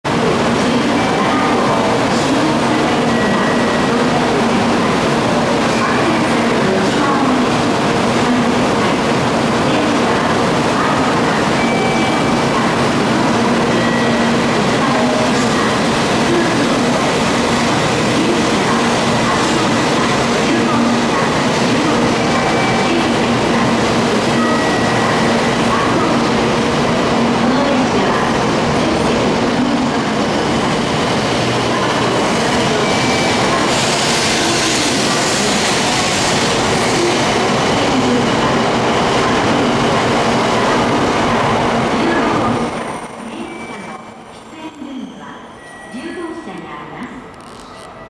Japan Trip - Shinkansen conductors
Japan really takes safety seriously.  Notice how many times they check to make sure no one will get caught by the train.  When they are looking up, they are looking at cameras that are views of where they can't see.